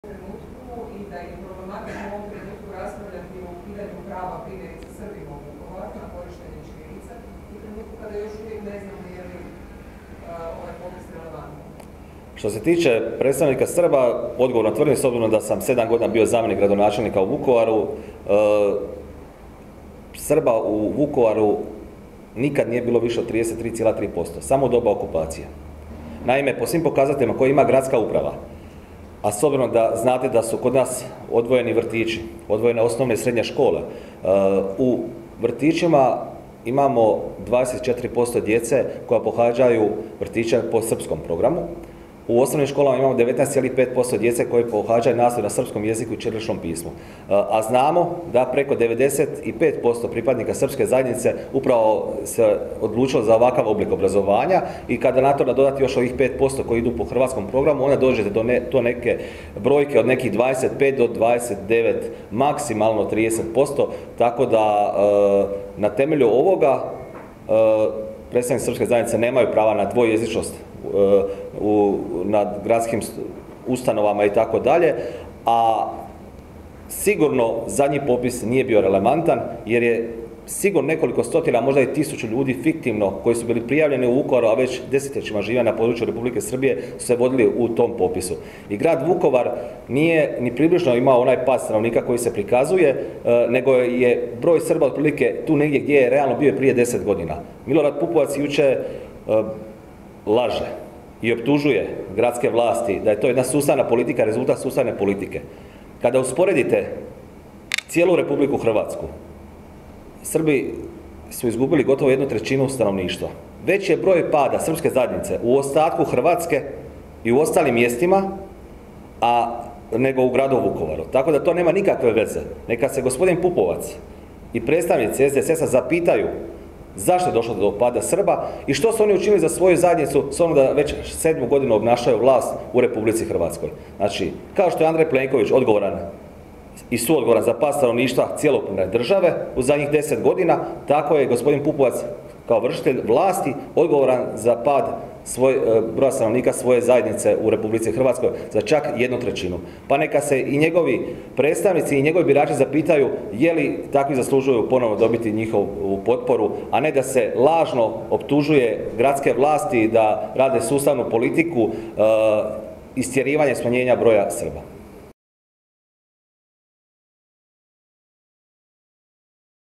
Pavliček: "Milorad Pupovac je kriv za pad broja Srba u Hrvatskoj"
Na konferenciji za medije održanoj u Saboru zastupnik Marijan Pavlićek govorio je o popisu stanovništva.